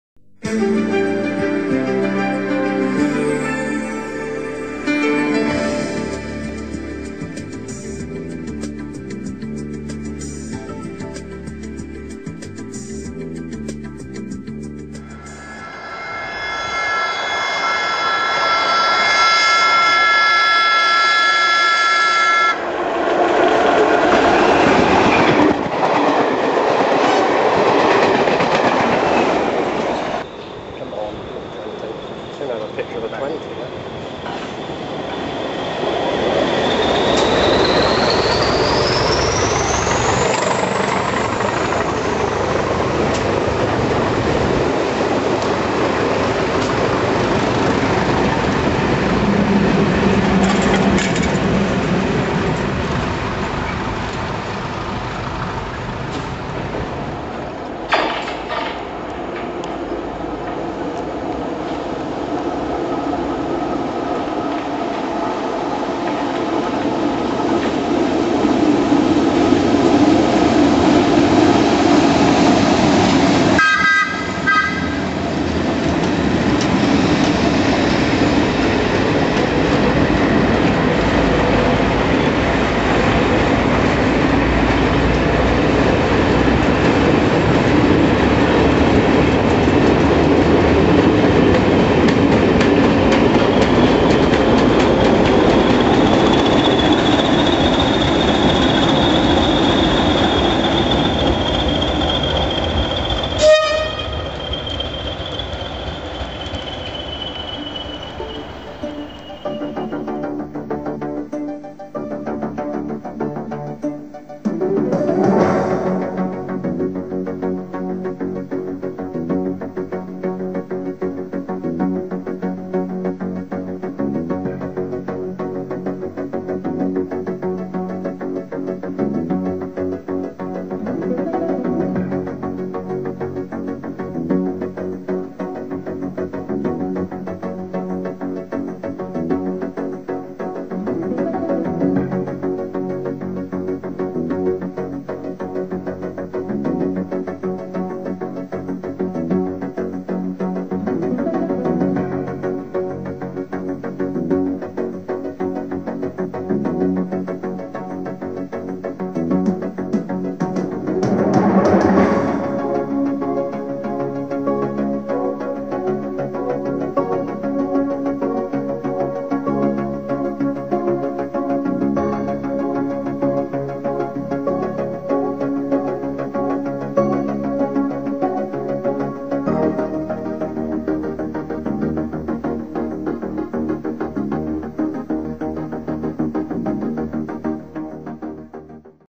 Metropolitan Heritage Special Sarah Siddons with 4TC & 20227
10th June 2007 Metropolitan Heritage Special The return of Sarah Siddons with 4TC & 20227.